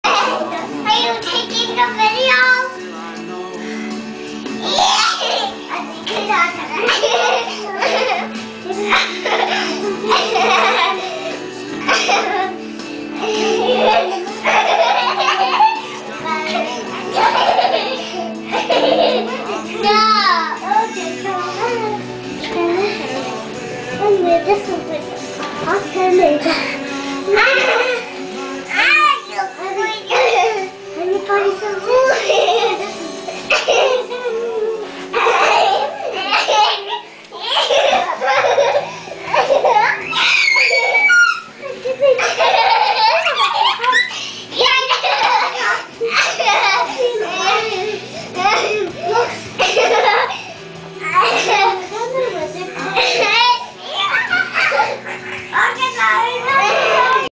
Wrestling--Aliyah & Caed
Rough housing with Aliyah & Caedmon